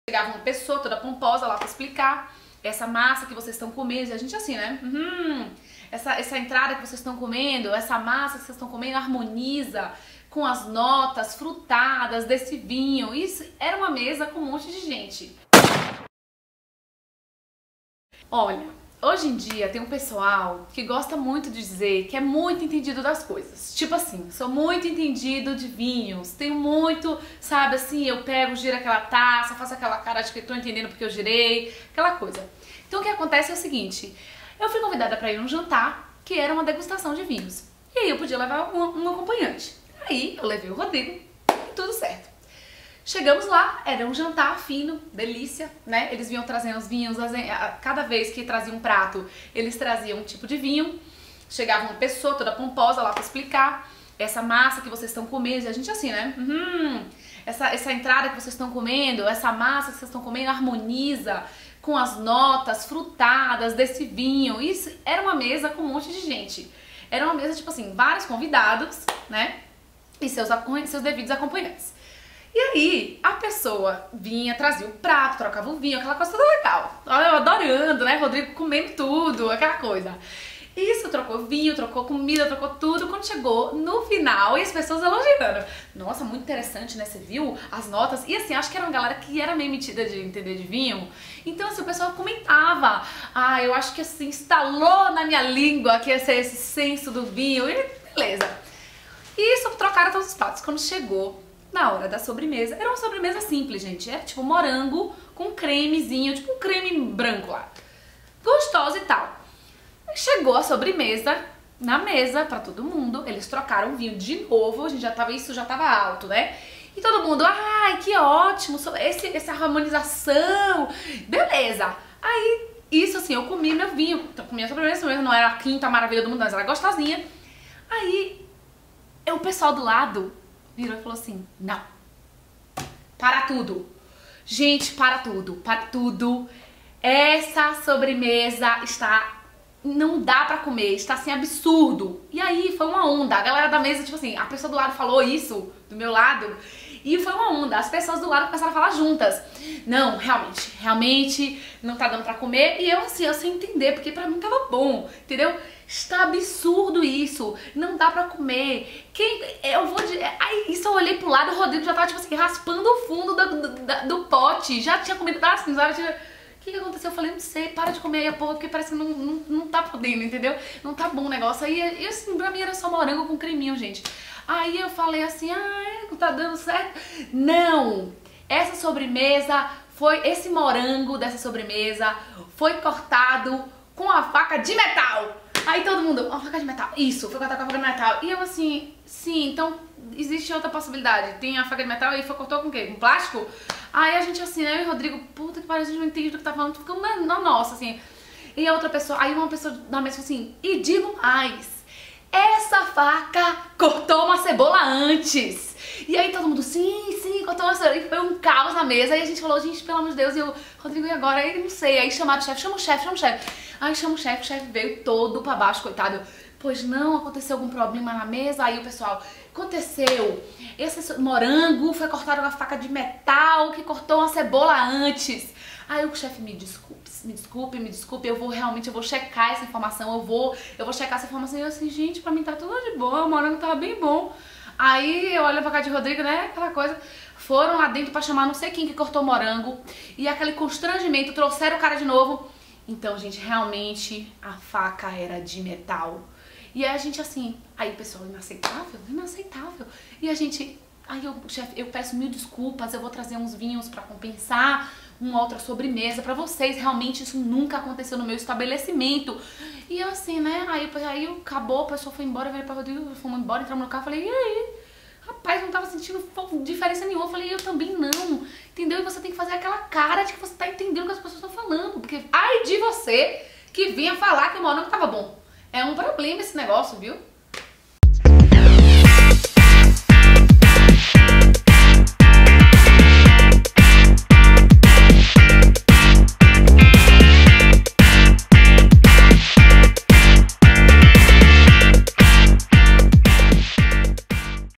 FACA DE CEBOLA | Camila Gaio
Pense na coisa mais esdrúxula que você conseguir imaginar. Sempre vai ter algum chato dizendo que é entendido do assunto. Degustador de vinho do Porto, expert de cerveja belga, apreciador de sorvete turco. Tem de tudo, até gente que reclama de boca livre.

"Faca de Cebola"

Novos vídeos todas terças e quintas às 10h.